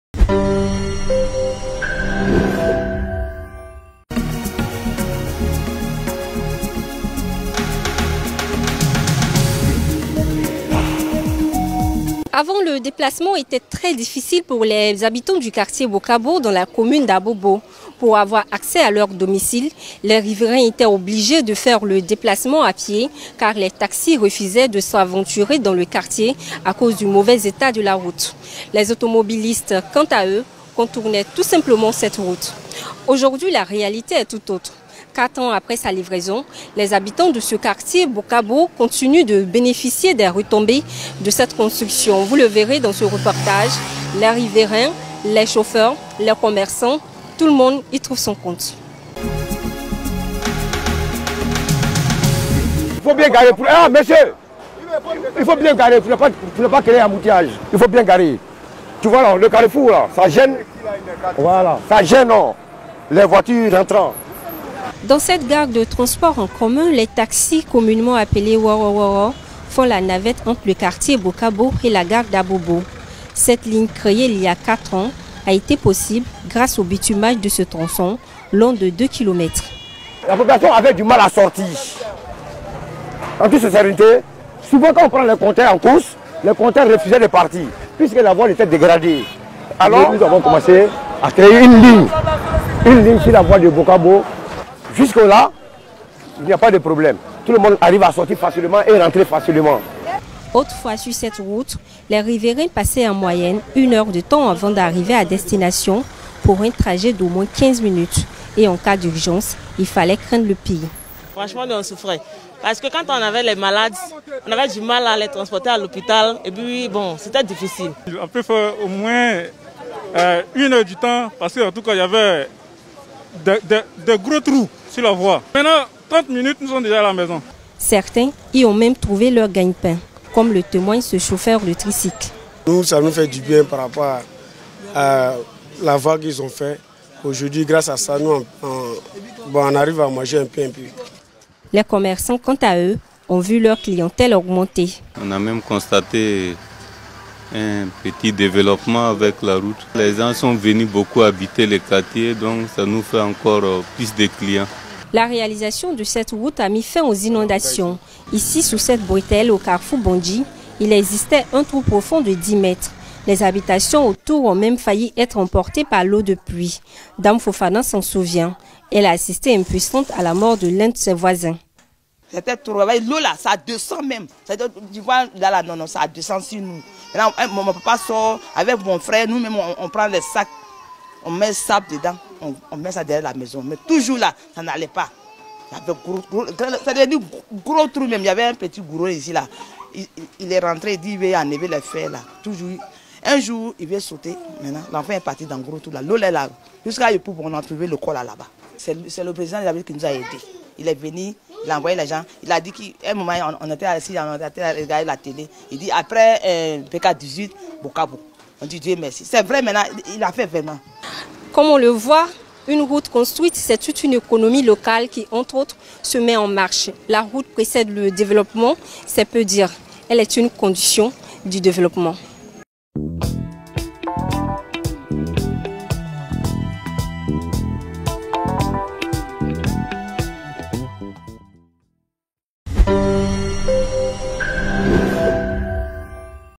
Le 8H/Abobo/ Bitumage de la route de Bocabo/Fin de calvaire pour les riverains.
Les travaux de voirie Bocabo-Abobo gare ont duré moins de deux ans. Depuis 2016, le bitumage de cette route signe la fin du calvaire des riverains. Son avènement a favorisé à la fois un développement local et économique.